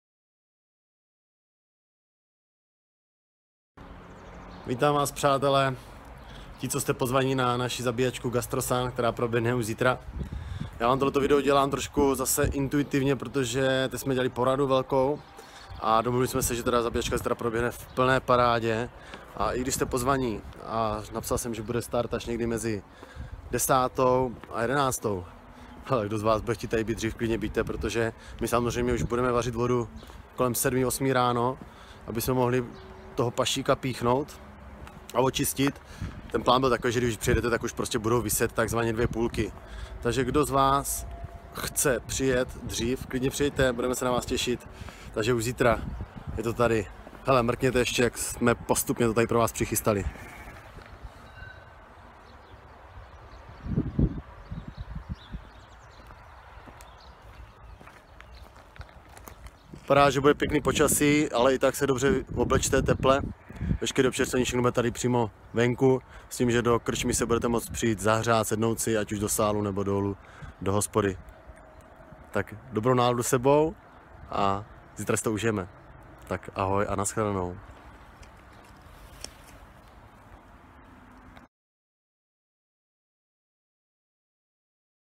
Gastrosun Zabíjačka 2017 zítra to vypukne
Už zítra se na Vás těšíme, tedy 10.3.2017 u nás na Krčmě a Hotelu U císařské cesty.